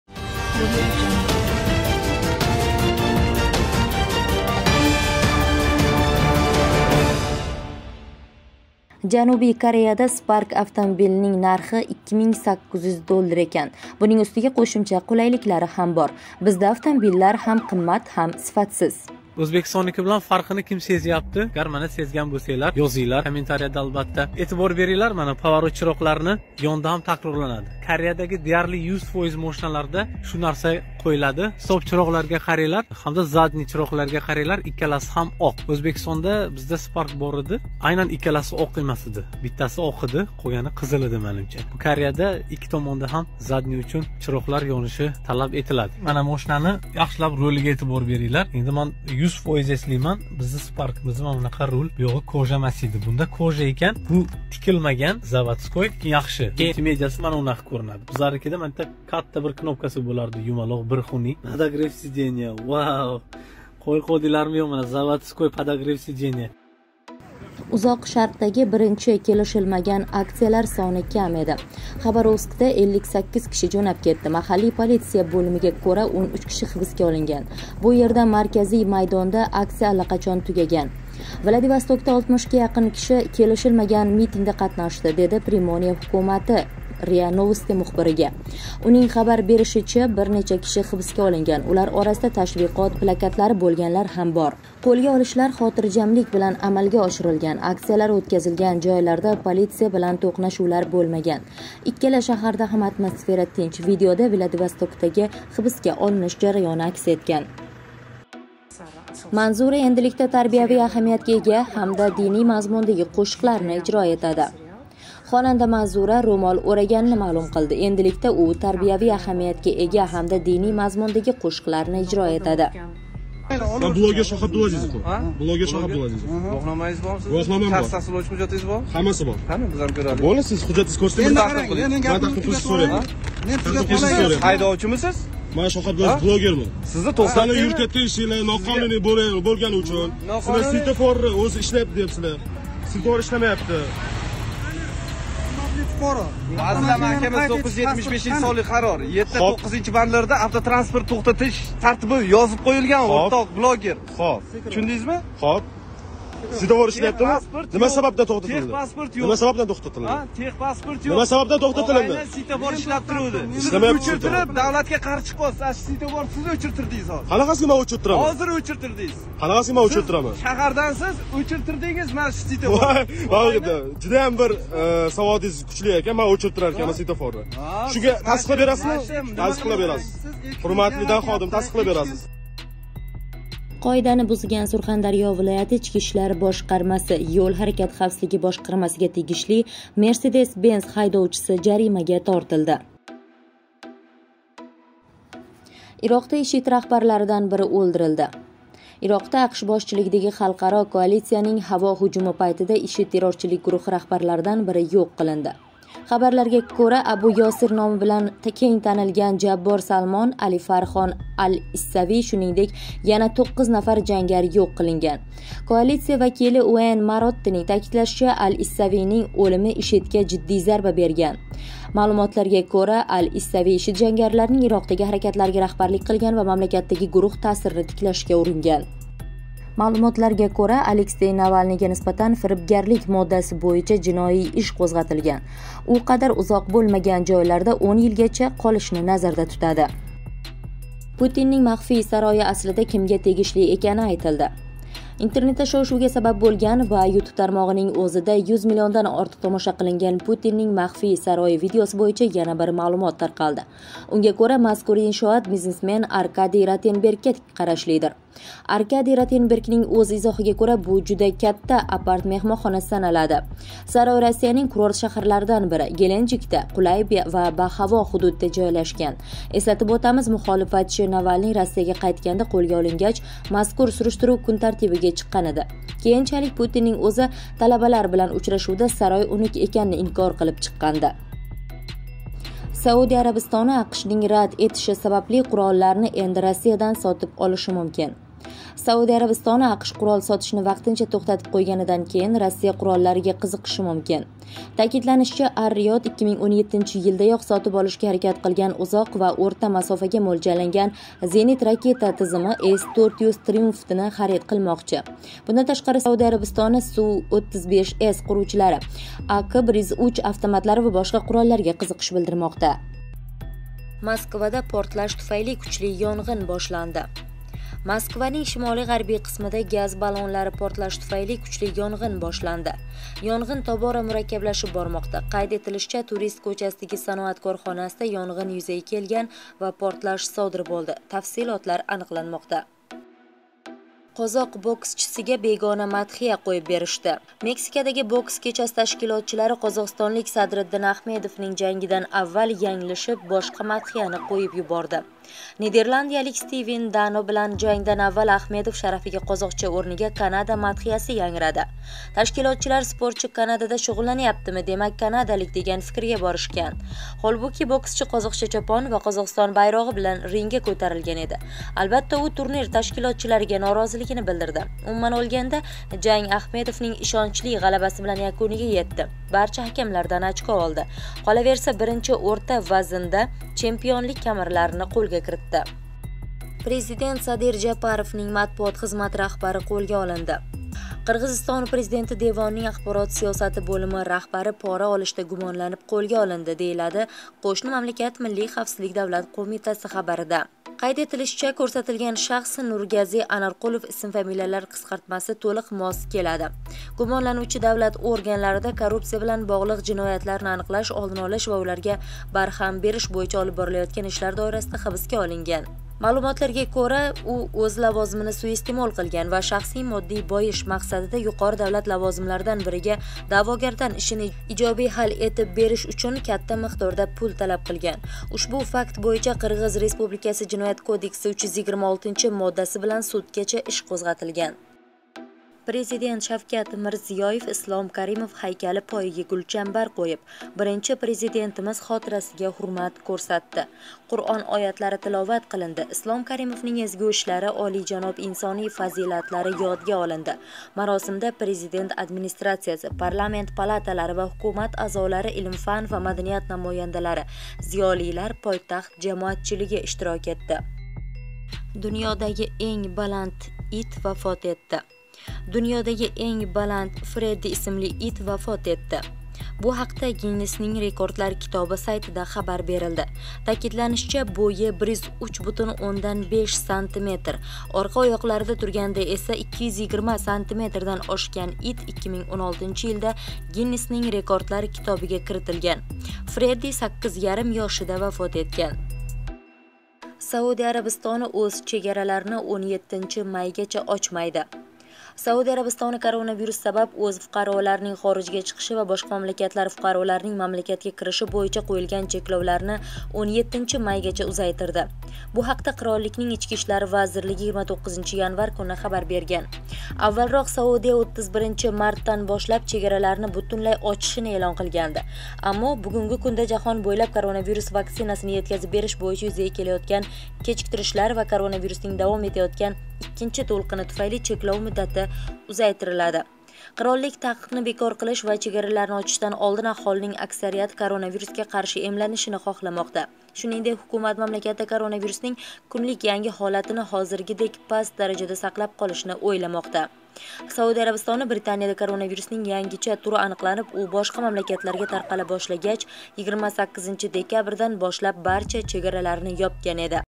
РОССИЯДА КУТИЛМАГАН ХОДИСА.ПУТИН МАХФИЙ СИРИ.УЗБДА СПАРК МОШИНАСИ.ШАВКАТ МИРЗИЁЕВДАН ЯНГИЛИК....
Президент Шавкат Мирзиёев Ислом Каримов ҳайкали пойига гулчамбар қўйиб, Биринчи Президентимиз хотирасига ҳурмат кўрсатди.
оятлари тиловат қилинди. Ислом Каримовнинг эзгу ишлари, олижаноб инсоний фазилатлари ёдга олинди.
Маросимда Президент Администрацияси, парламент палаталари ва ҳукумат аъзолари, илм-фан ва маданият намояндалари, зиёлилар, пойтахт жамоатчилиги иштирок этди.
Президент Шавкат Мирзиёев почтил память Ислама Каримова, возложив цветы к подножию памятника Первому Президенту в городе Ташкенте.
Прочитаны суры из священного Корана. Говорилось о благородной деятельности, высоких человеческих качествах Ислама Каримова.
В церемонии возложения цветов приняли участие представители Администрации Президента, члены парламента и правительства, деятели науки и культуры, творческая интеллигенция, общественность города.